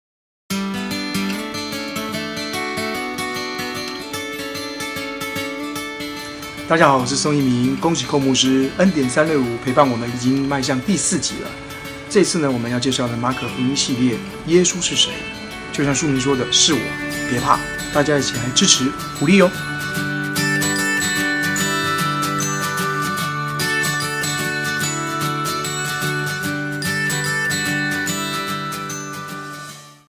新書 - 是我別怕 名人推薦篇 : 宋逸民
影片版權由 天聲傳播協會 所有

這本書要介紹一個很特別的人，
他總在關鍵時刻出現，
他一出現，就帶來一份奇特的力量，
安定的力量，平靜的力量！
他，到底是誰呢？

非買不可的三大理由：
1. 身臨其境，體驗神蹟奇事！
2. 人生關卡，給你最佳選擇！
3. 隱藏版祝福，只要相信，立即擁有！